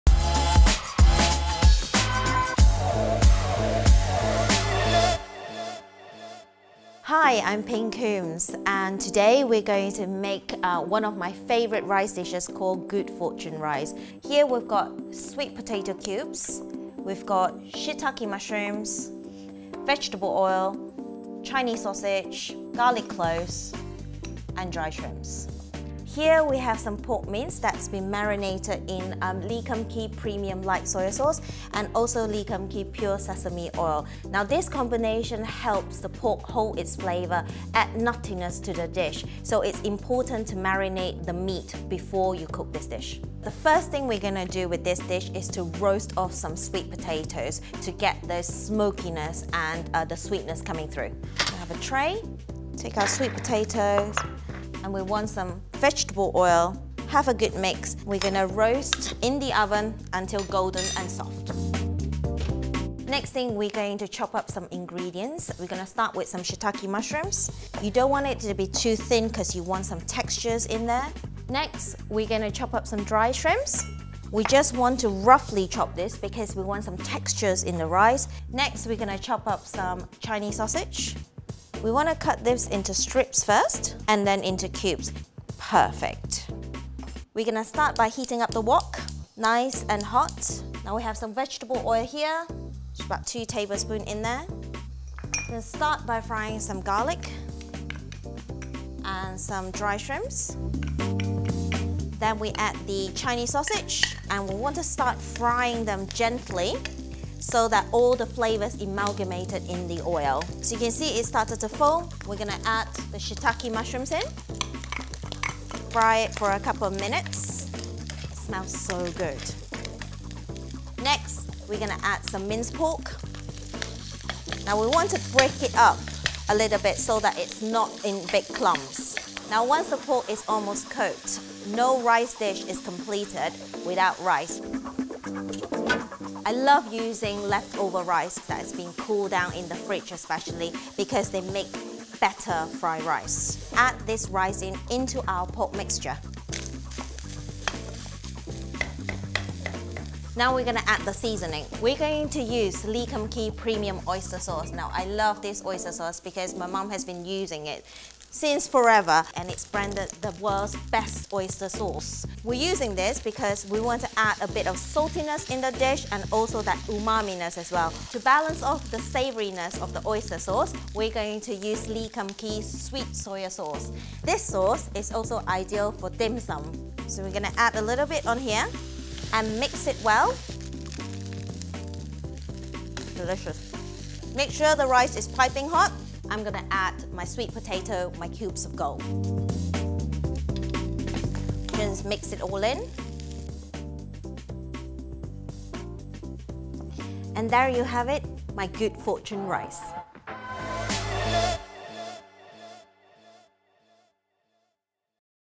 Love Home Taste - Chinese New Year - Ping Coombes' Good Fortune Rice
- See more form our chefs here.

Ping Coombes cooks up one of her mother's Chinese New Year dishes with a few changes and additions. Rustle up a big batch of this Good Fortune Rice for all your family and friends.

"In Chinese symbolism rice represents good fortune. When I was younger, my mum always encouraged me to finish every grain of rice in my bowl, she said if I don’t I will get a very spotty husband. I was so terrified that I would never leave my rice unfinished! I am happy to say I have a spotless husband who loves me unconditionally so I guess it is due to my good fortune and my impeccable rice eating.
I have created a variation of my mum’s yam rice by using a more available ingredient, sweet potato, to symbolise cubes of gold and to add little bursts of sweetness. This dish is flavoured with pork, dried shrimps, light soy sauce and oyster sauce to create mouthful of taste sensations. I also top it with an egg yolk, dressed with Lee Kum Kee Chiu Chow Chilli Oil to add richness to the rice." - Ping Coombes

Serves 4

Ingredients:

- 200g pork mince
- 2 tbsp. Lee Kum Kee Premium Oyster Sauce
- 1 tbsp. Lee Kum Kee Sweet Soy Sauce
- 200g sweet potatoes, peeled and diced into 1 cm cubes
- 2 tbsp. vegetable oil
- 25g dried shrimp (optional, use fresh shrimps as an alternative)
- 2 garlic cloves, chopped
- 1 Chinese Sausage, diced (use chopped salami or pancetta as an alternative, around 100g)
- 6 dried shitake mushrooms, rehydrated in warm water for around 30 minutes, until softened before use or use fresh shitake mushrooms
- 400g cooked rice
- 2 spring onions, finely sliced
- 1 long red chilli, finely sliced

Marinade for the pork:

- 1 tbsp. Lee Kum Kee Premium Light Soy Sauce
- ¼ tsp. Lee Kum Kee Pure Sesame Oil
- Pinch of white pepper
- ¼ tsp. caster sugar
- ½ tbsp. cornflour

Egg Yolks (Optional)

- 2 egg yolks
- 1 tsp. Lee Kum Kee Chiu Chow Chilli Oil

Method

1. If curing the eggs, mix the soy sauce, chilli oil and sesame oil in a small container with lid. Carefully place the egg yolks on top of the mixture. Leave in the fridge for 4-5 hours, a hardened film will form on top of the yolk. It will also look smaller in size.

2. Preheat oven 180C /160C Fan.

3. Mix together the marinade for the pork, add the mince then cover and leave to marinate in the fridge while you prepare the rest of the ingredients.

4. Roast the sweet potatoes in 1 tablespoon of oil for 25-30 mins. Remove and cool.

5. Re-hydrate dried shrimps in hot boiling water for 5 mins, remove water and chop.

6. In a wok, heat the remaining oil on medium heat. Add garlic, dried shrimps and Chinese sausage and sauté for about 2 -3 mins until fragrant and the mixture starts to foam. Add mushrooms and sauté for another 2 minutes.

7. Add marinated mince pork and fry for 5 mins. Break the pork up so it is not in big clumps. Stir in the roasted sweet potatoes.

8. Add oyster, dark and sweet soy sauce. Add rice and mix well. Stir fry for another 5 minutes so that the rice is piping hot and it is well mixed with the meat.

9. Top with egg yolks and dress it with Lee Kum Kee Chiu Chow Chilli Oil. Sprinkle with sliced spring onions and chilli.